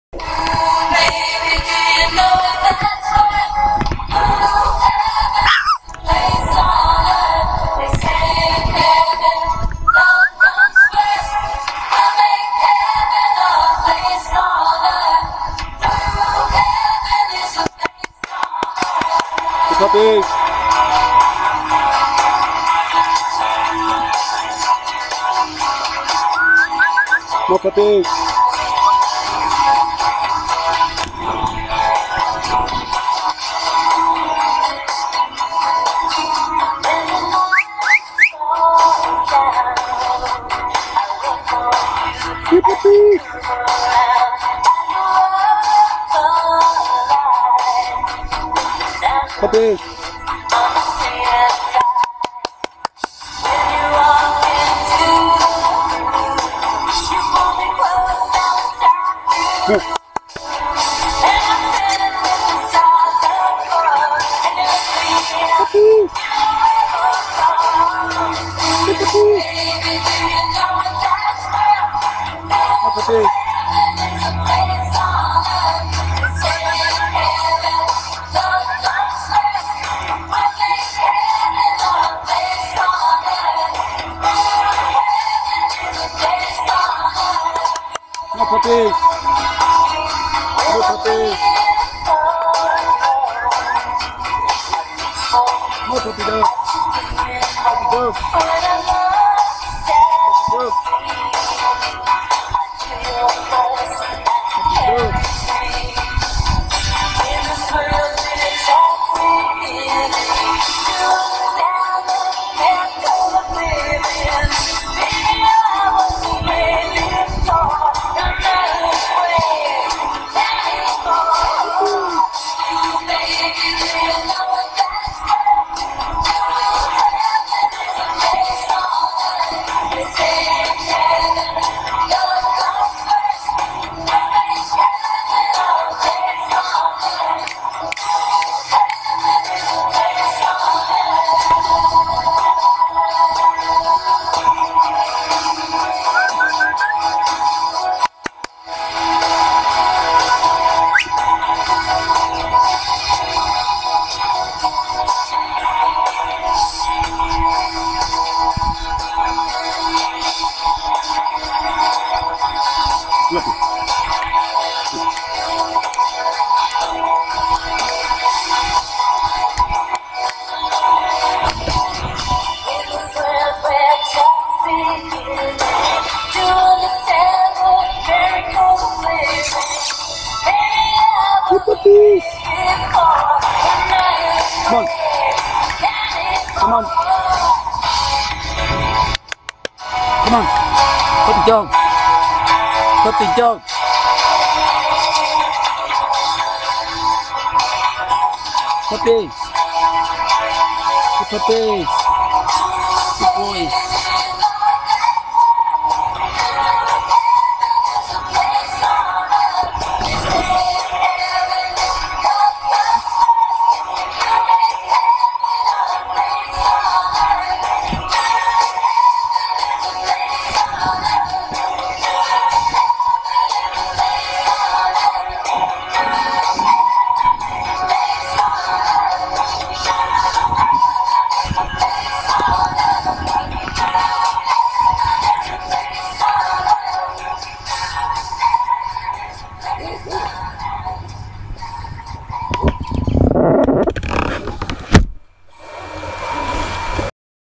Max Muscle x Ms.Olympia puppies
Max Muscle X Ms. Olympia
Muscle Mastiff puppies born Easter Sunday 2015
500.00 each